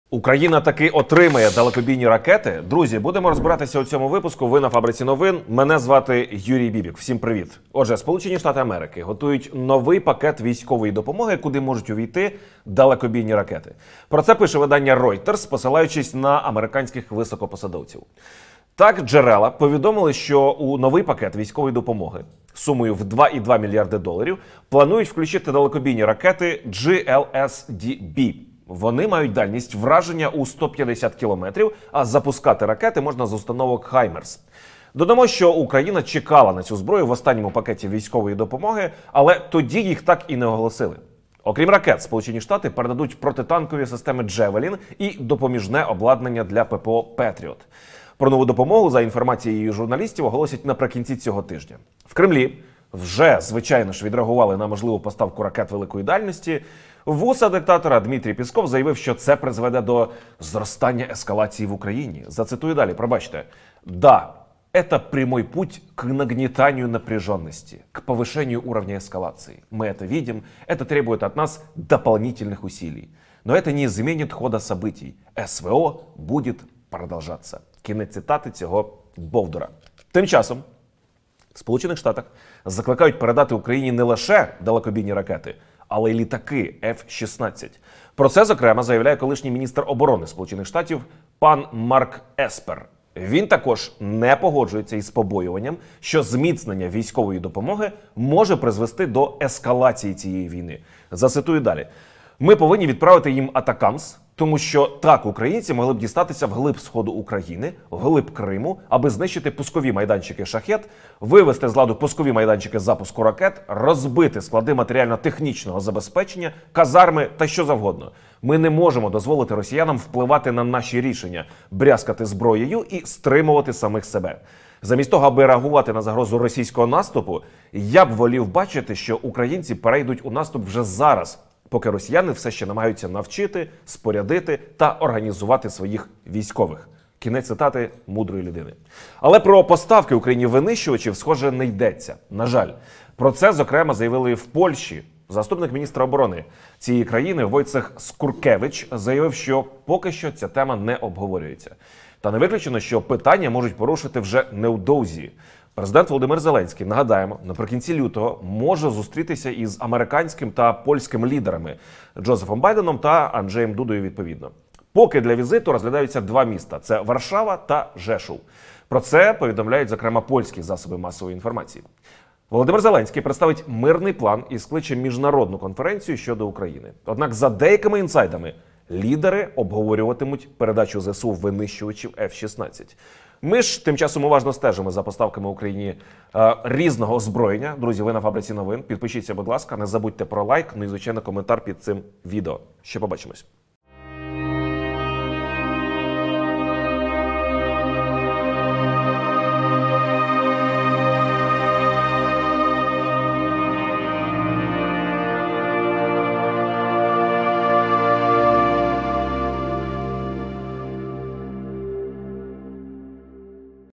🚀ПУТІН БЛАГАВ ЦЬОГО НЕ РОБИТИ! США вирішили ПЕРЕДАТИ ЗБРОЮ для ударів по ТИЛУ РОСІЯН
Американські ЗМІ пишуть про те, що Білий дім вирішив переступити червоні лінії, які озвучив російський диктатор Володимир Путін та передати ракети великої дальності для ЗСУ. Ця зброя дозволить вражати цілі у тилу сходу та півдня України, зокрема й у Криму. Подробиці дивіться у цьому ролику.